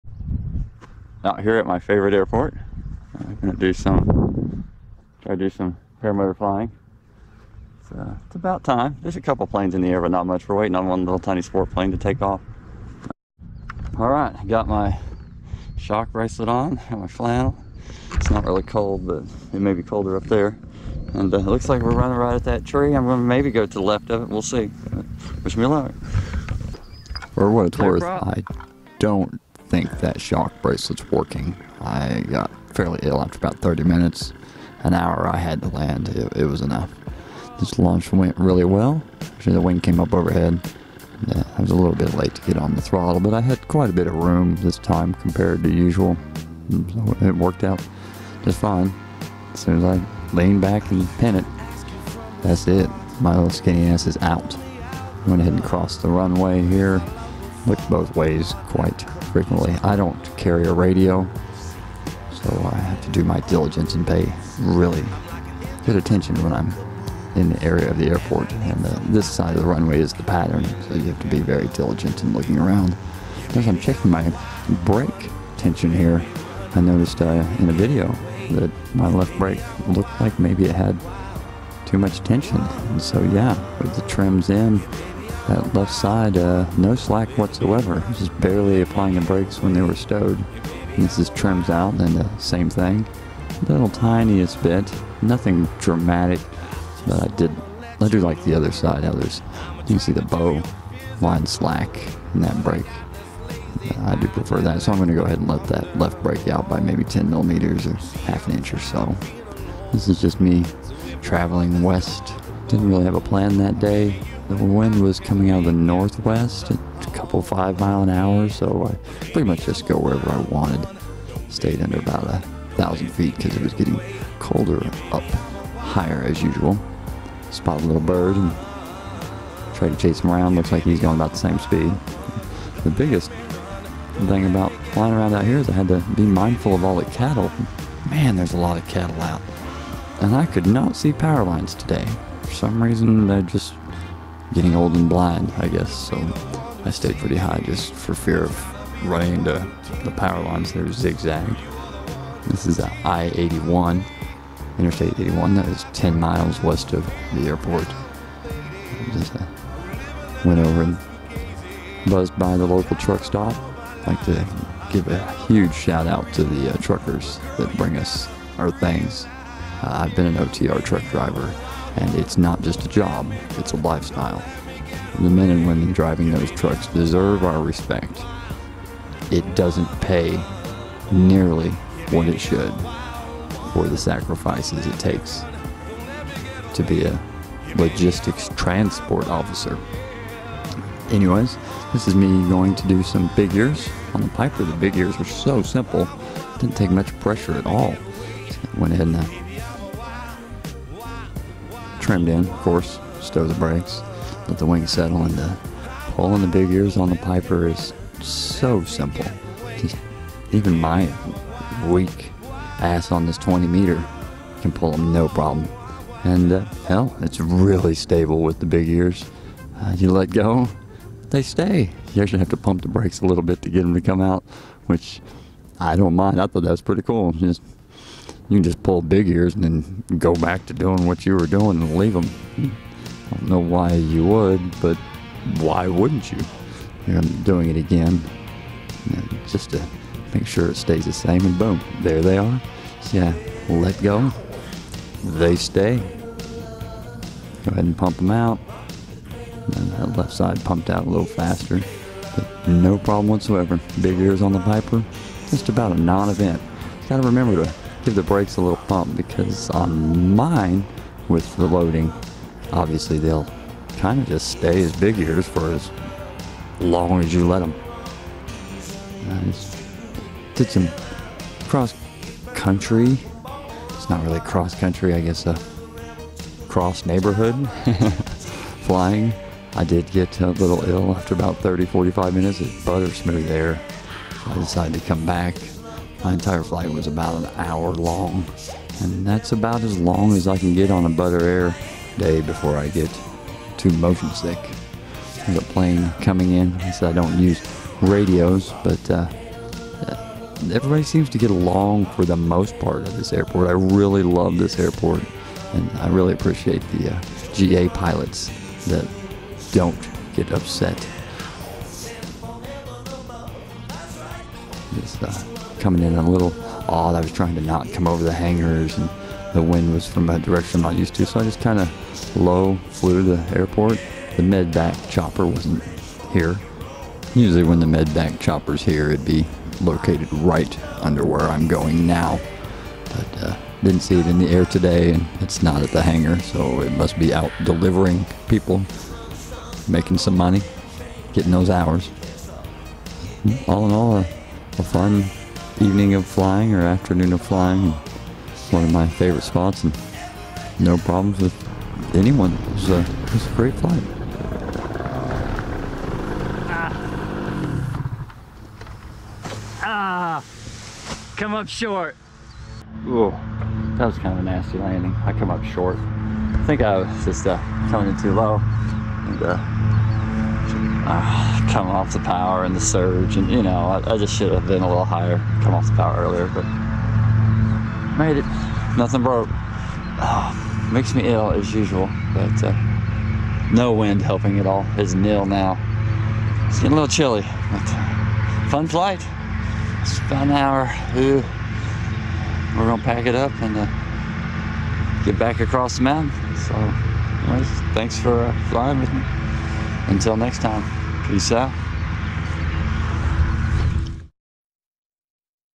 Paramotor flight in East TN
Went for another hour flight from my favorite airport. Took up my 20m piper, ran a 20 mile loop and did a couple Big Ears.. Fun times...